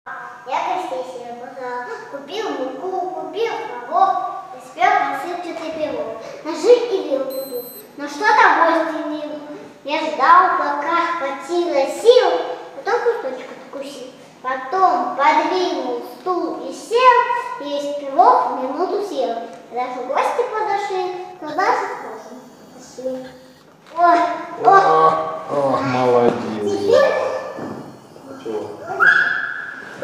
Растяжка в игровой манере с детьми младшего возраста. Мурманск. Кёкусинкай
Индивидуальная детская тренировка 21 февраля 2017 года
Гимназия №3, ул. Челюскинцев 14-а